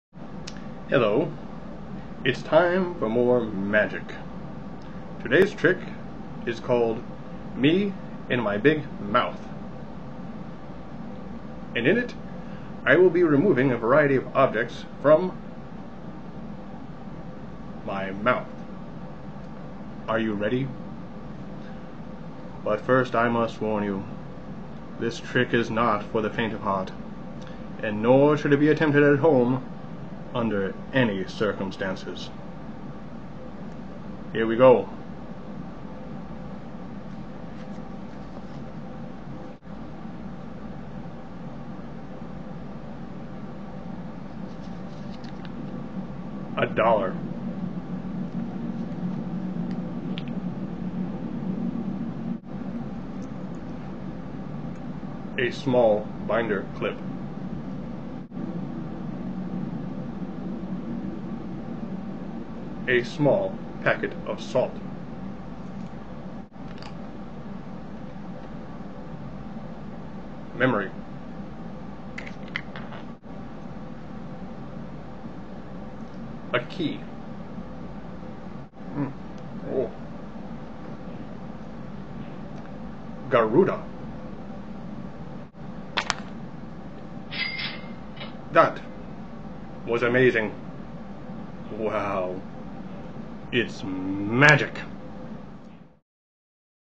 Me and My Big Mouth
I magically produce a variety of objects from my mouth